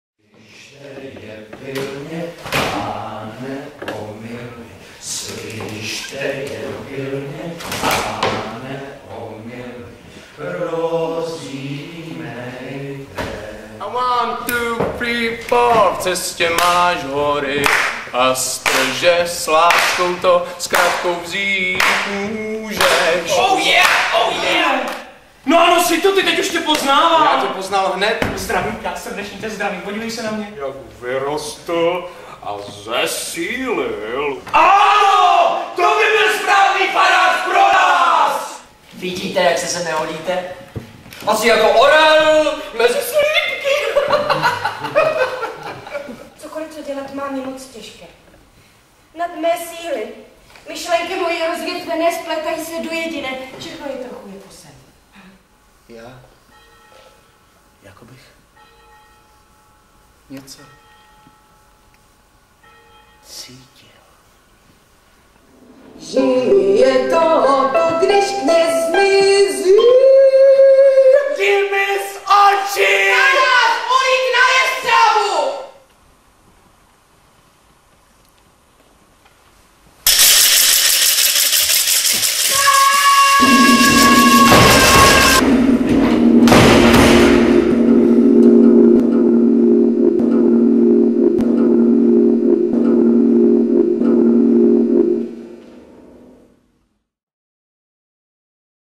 Henrik Ibsen: Brand-Ohen (trailer)
Henrik Ibsen
Brand -- Ohen
Klenute mysterium s prvky bystre frasky

"Režisér Pácl pracuje se slovy s obrovským respektem k jejich obsahu,  ale inspiruje se i zvukomalebnými možnostmi textu - vidí `verš jako prastarý vnitřní rytmus příběhu promítnutý do obrazové i zvukové rezonance inscenace`."

23. zari 2010 od 10.00 v Divadle v Celetne
26. zari 2010 od 19.30 ve Studiu Alt@ - Hala 30
29. rijna 2010 od 19.30 v Divadle v Celetne
14. listopadu od 20.00 ve Studiu Alt@ - Hala 30
19. prosince od 19.30 ve Studiu Alt@ - Hala 30

Hrají: Miloslav König, Lucie Štěpánková, Pavla Beretová, Jan Meduna, Jiří Panzner, Radovan Klučka, Richard Fiala, Václav Jelínek

Režie: Štěpán Pácl

Studio Alt@ - Hala 30 (U výstaviště 21, Holešovice, Praha 7 / Spojení: tram č. 5, 12, 14, 15, 17 zastávka Výstaviště, ve skladovém areálu naproti Výstavišti v Hale 30, vchod do areálu černou branou mezi autobazarem a obytným blokem, web